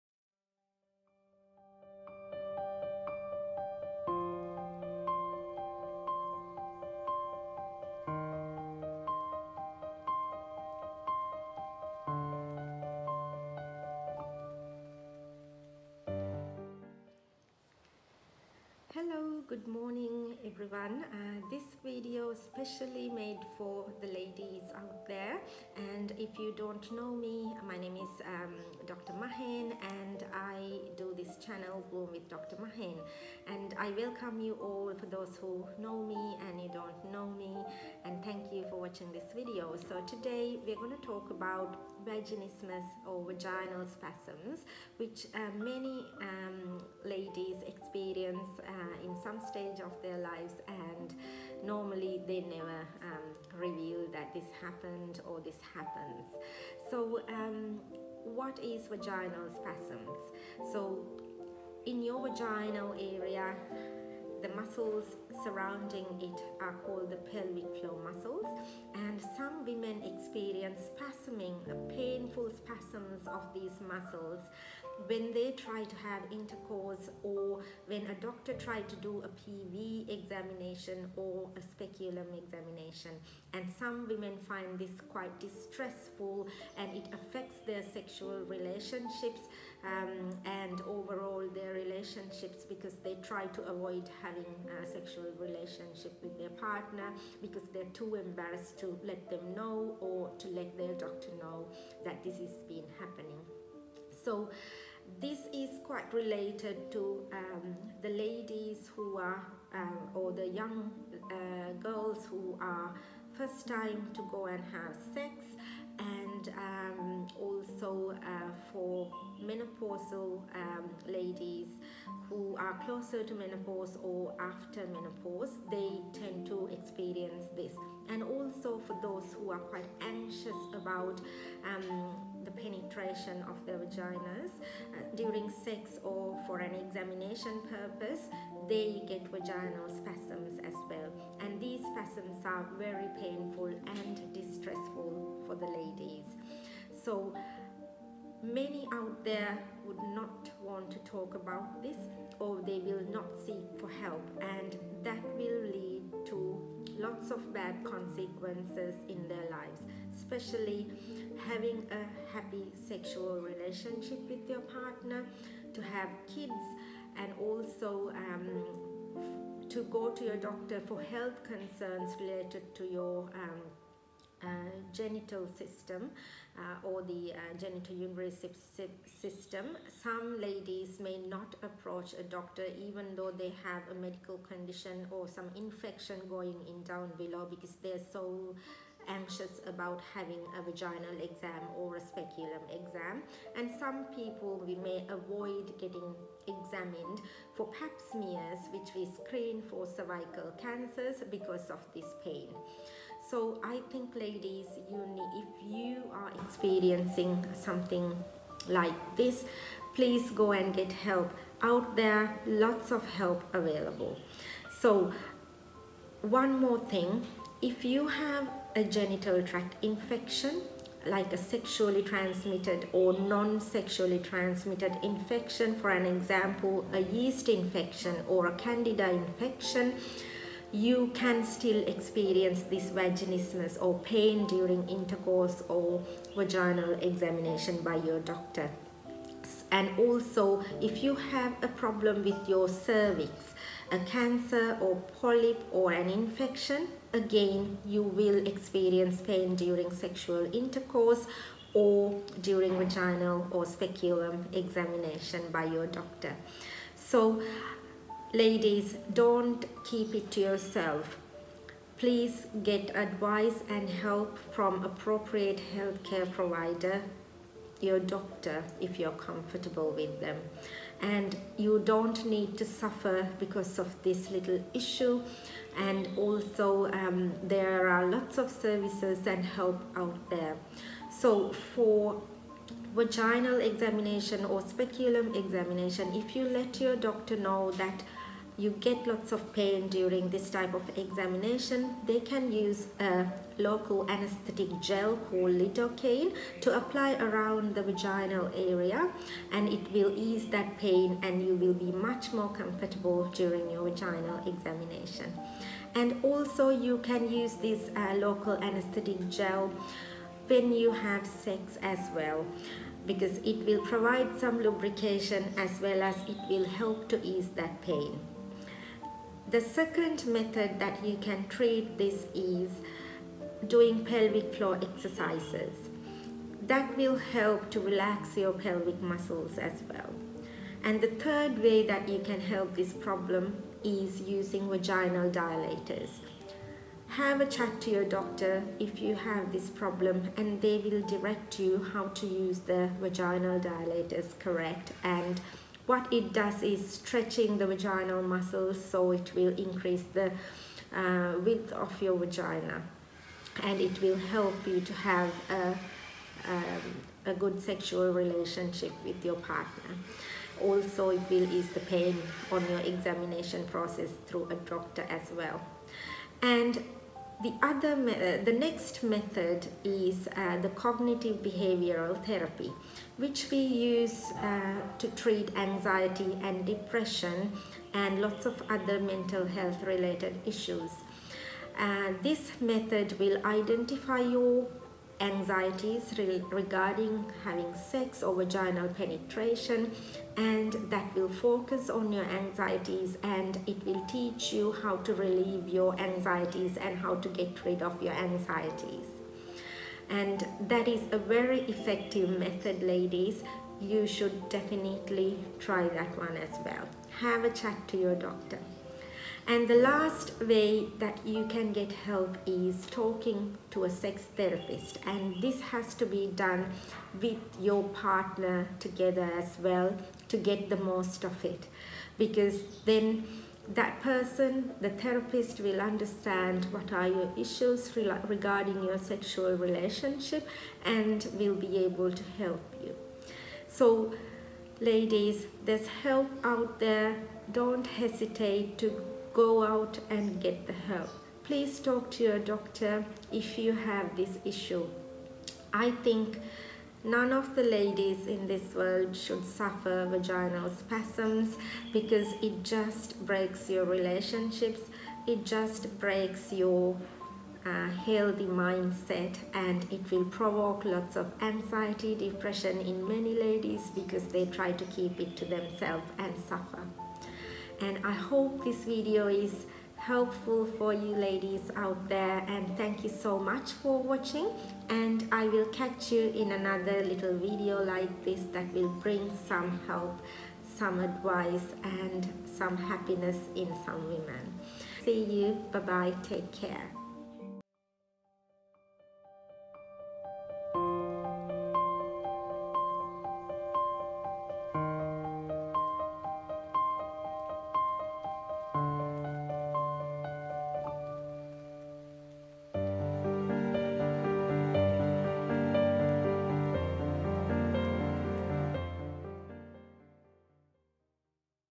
How to overcome painful vaginal spasms in 2023 ?
hello ladies all over the world ! this video is specially for those you have suffered and currently experiencing painful spasms of your vagina during intercourse, vaginal or speculum exam by your doctor .
Ladies this can be treated and you do not have to suffer !!!